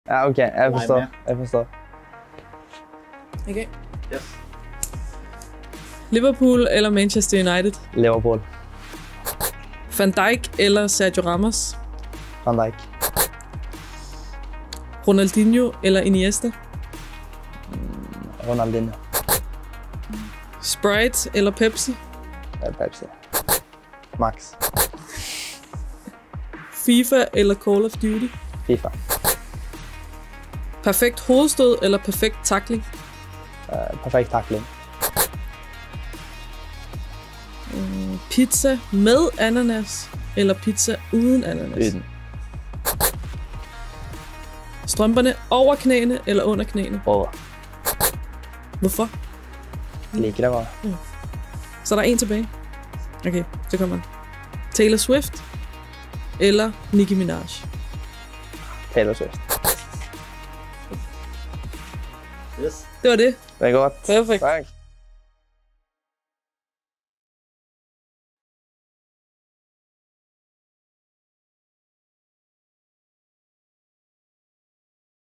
9 HURTIGE TIL HENRIK HEGGHEIM
Så blev det Henrik Heggheims tur til at svare på 9 hurtige.